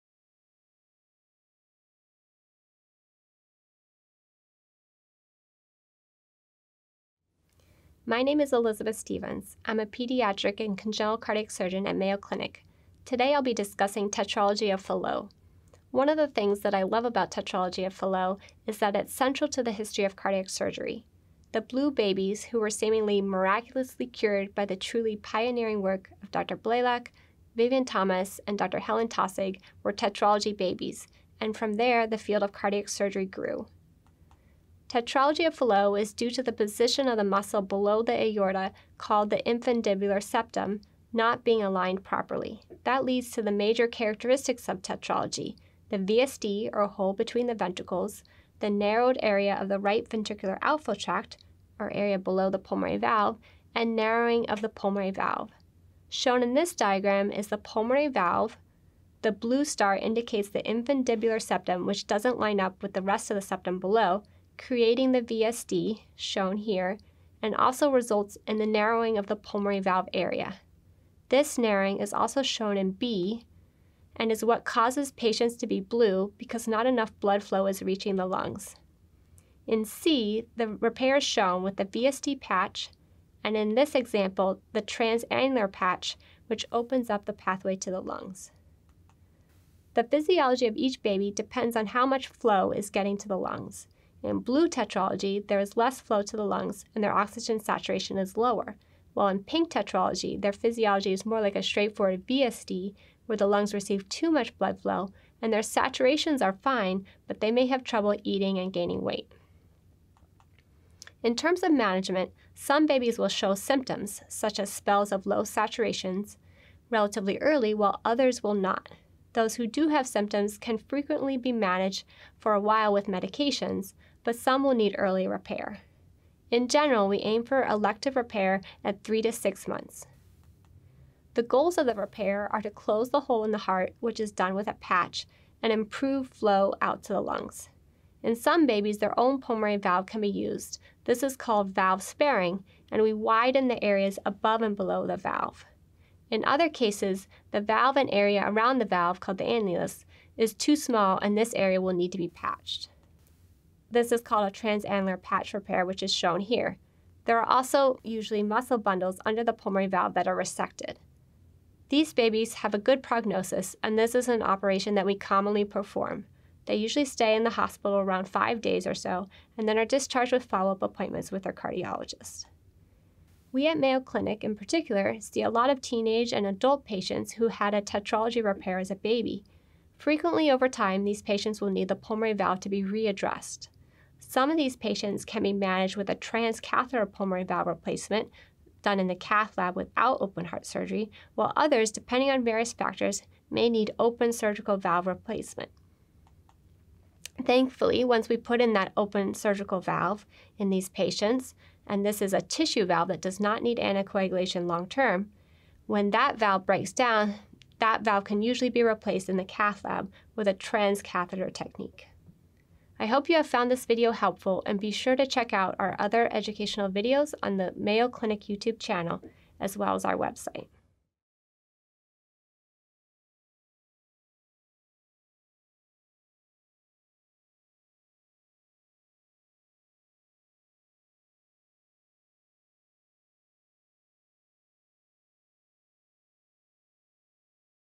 Mayo Clinic – Tetralogy of Fallot
In this video, Elizabeth H. Stephens, MD, PhD discusses the anatomy, physiology, and surgical management of tetralogy of Fallot. Tetralogy of Fallot has a constellation of findings including a ventricular septal defect and often a narrowed pathway to the lungs which can cause babies to be “blue.”  While the first surgical repair is commonly performed in infancy, these patients frequently require reoperation on the pulmonary valve.  These topics are expounded upon in the video. Dr. Stephens is a pediatric and congenital cardiac surgeon at the Mayo Clinic.